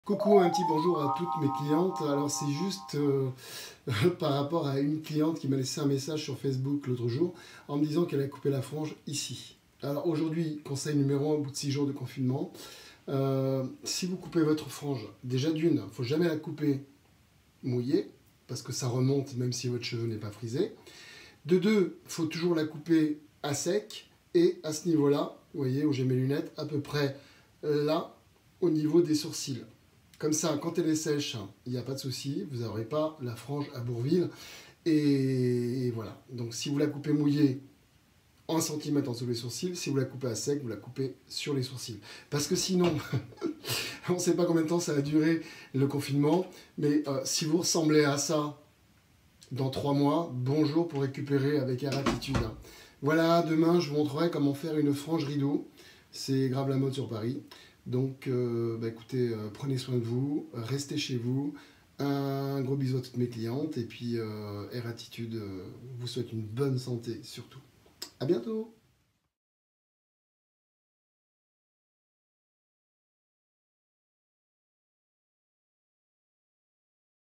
Comment se couper la Frange tout seul
R'ATTITUDE salon de coiffure a Viarmes dans le val d'Oise
. Conseil N°1 la frange au top tout seul